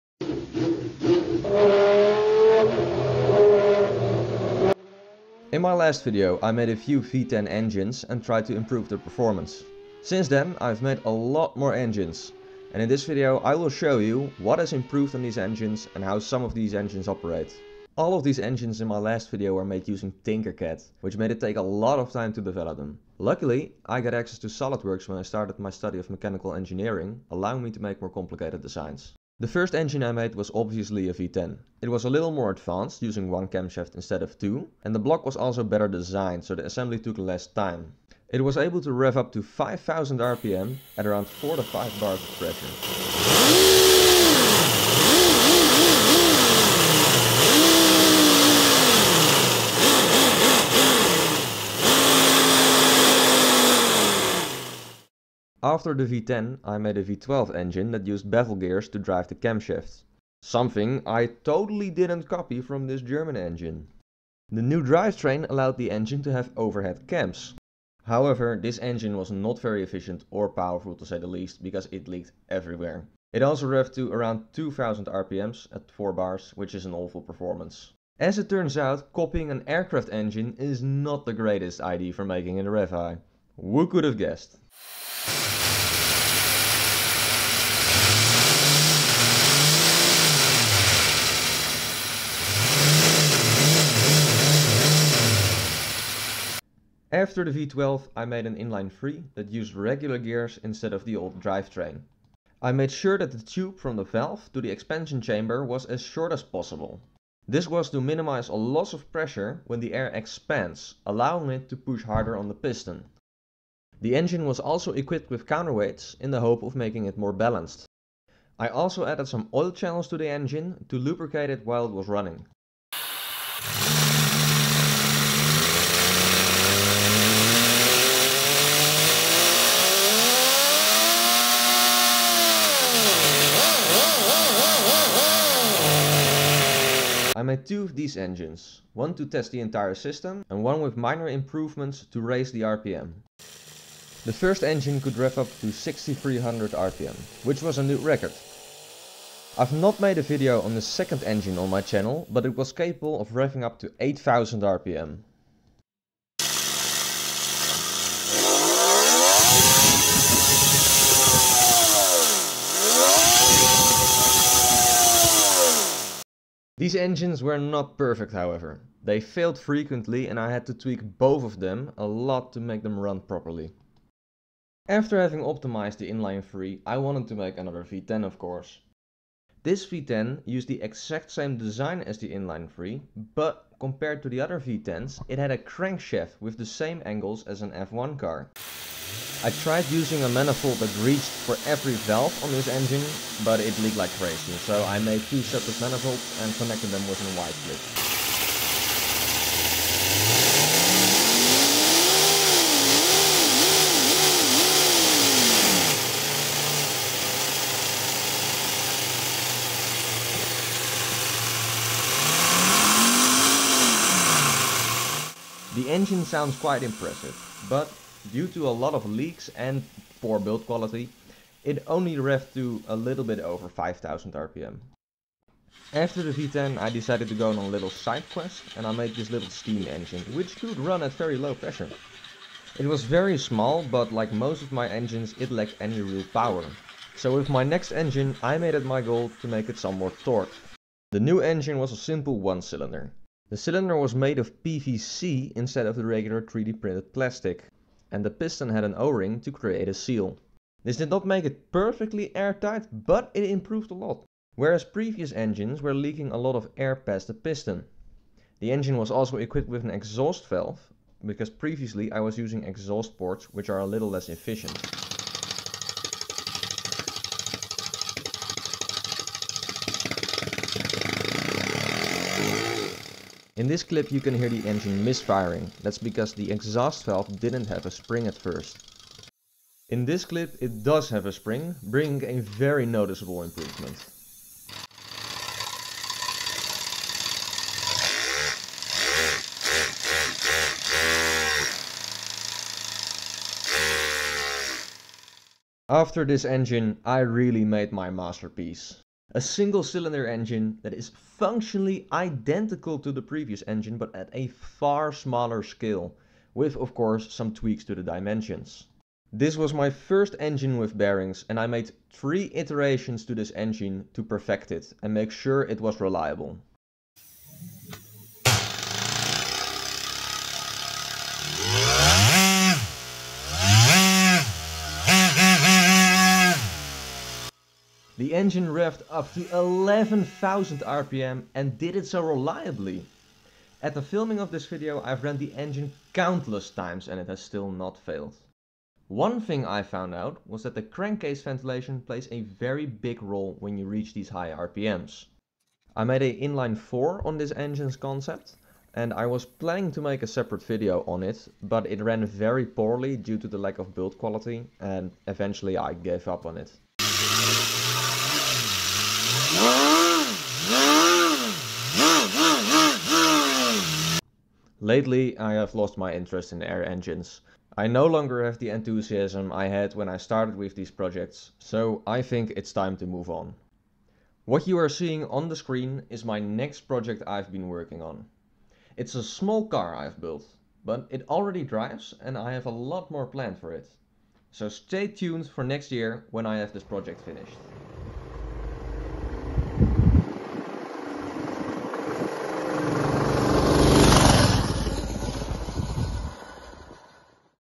Improving 3D Printed Air Engine RPM
In this video i show the air engines i have made and how i improved them. Going from 5000 rpm to over 11000 rpm.

This is my last air engine video as i have lost interest in air engines, but i have more projects so more videos will follow in the future! Thanks everyone for tuning in to my engine projects!